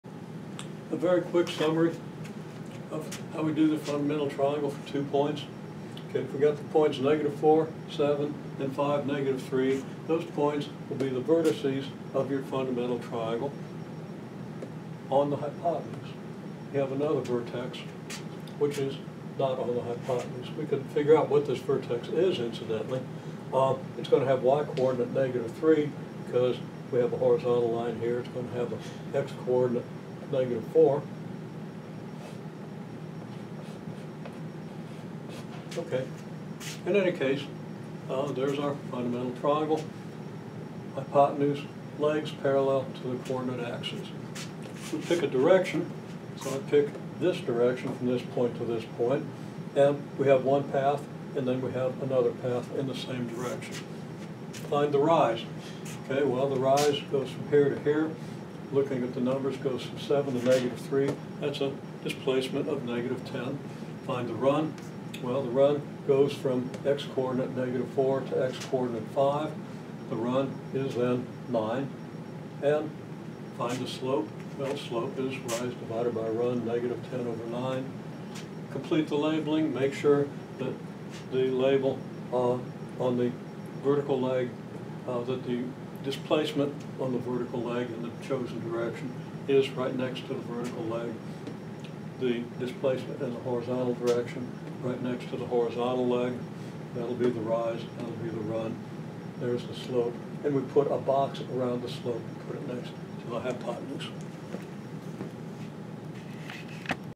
dagpcb 4792 fundamental triangle for two points brief 2
Brief summary of #4791, Fundamental Triangle for two points.  We construct and label a Fundamental Triangle for two points.